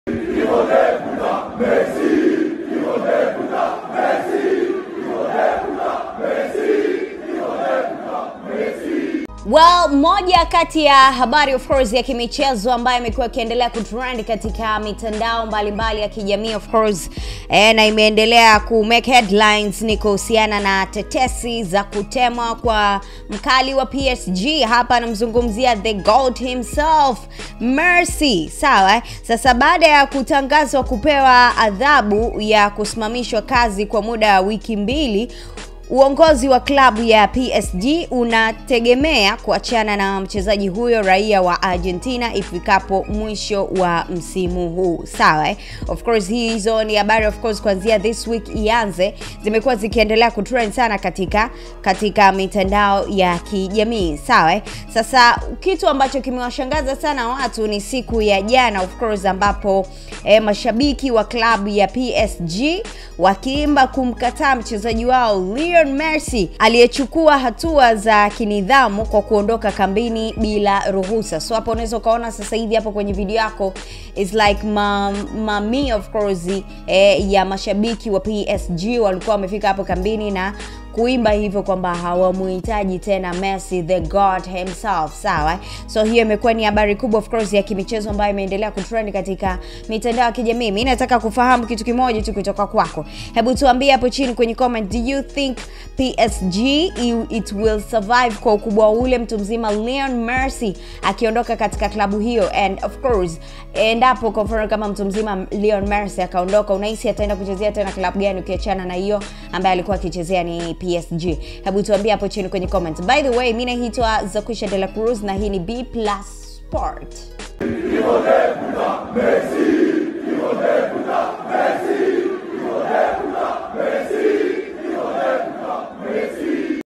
MASHABIKI WA KLABU YA PSG WAMKATAA MESSI WAANDAMANA WATAKA AFUKUZWE KISA UTOVU WA NIDHAMU...
WeAreEverywhere